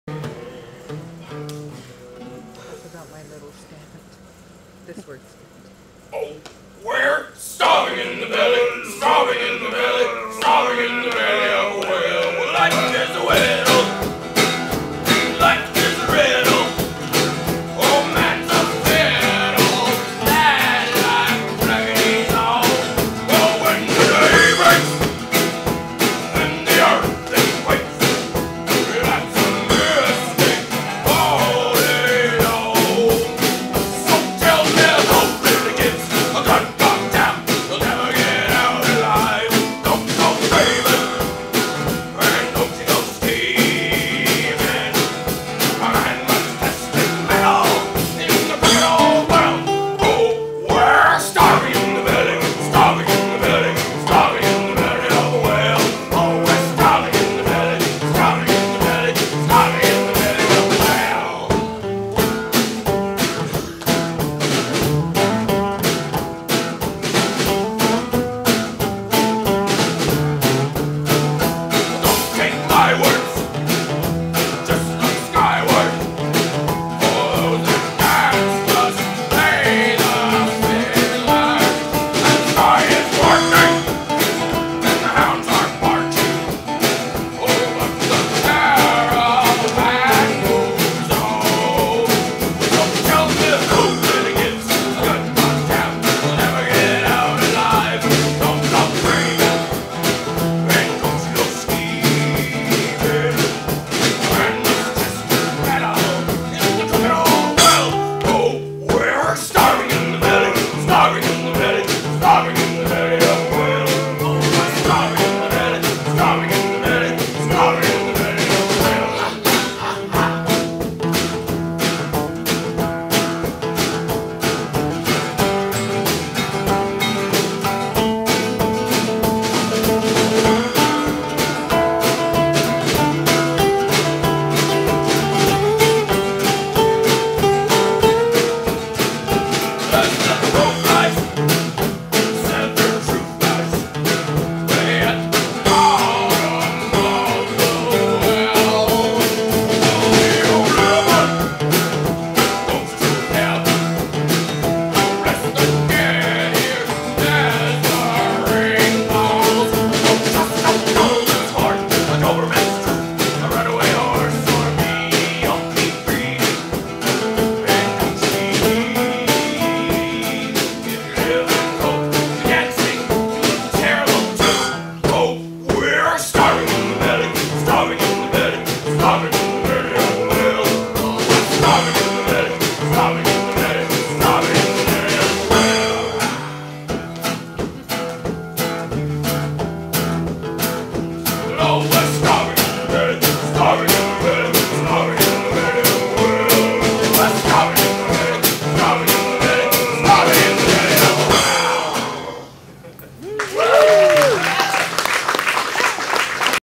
Otter Storm Downtown Dinner Hour
Debut gig at Subcity Donair Salmon Arm Friday May 3 Starvining in the Belly of a Whale